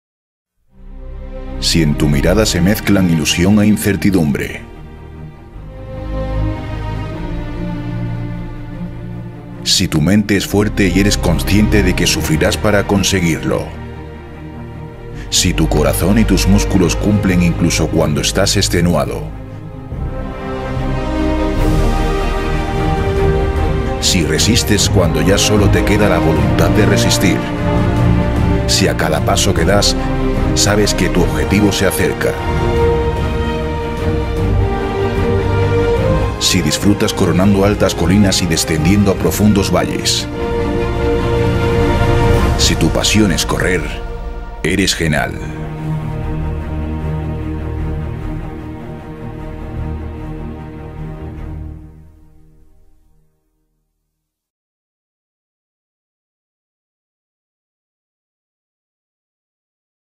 AVANCE VÍDEO OFICIAL GRAN VUELTA VALLE DEL GENAL 2016
Os dejamos un avance del vídeo oficial de la UTGVVG 2016 que se celebró durante los días 28, 29 y 30 de octubre y que contó con la participación de más de 1.000 deportistas.